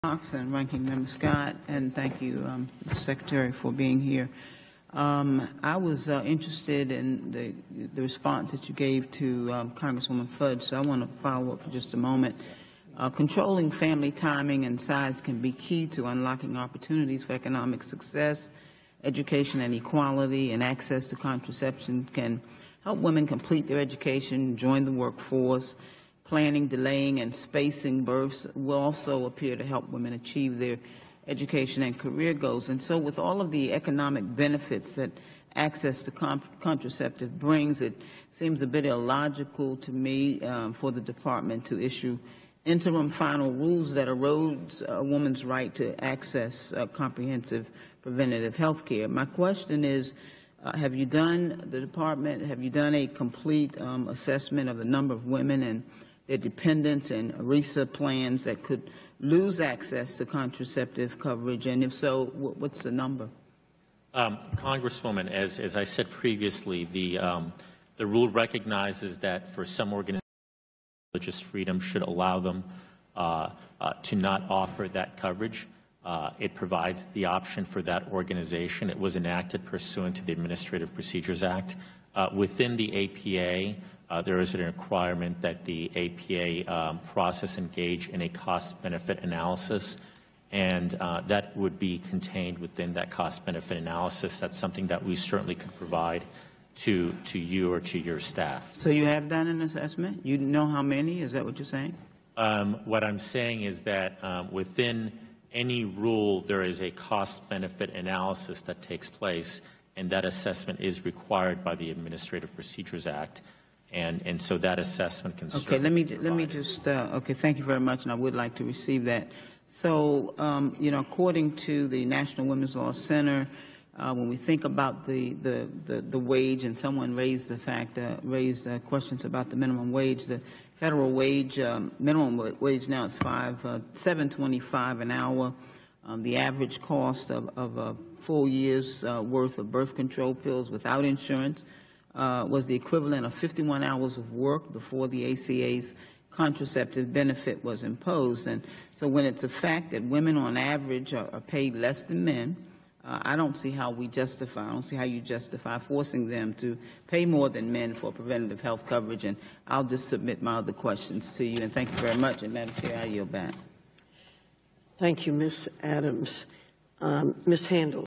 Rep. Alma Adams Stands Up for Women at Acosta Hearing
Ranking Member Alma Adams (NC-12) held Secretary Acosta accountable for the Trump administration anti-worker agenda.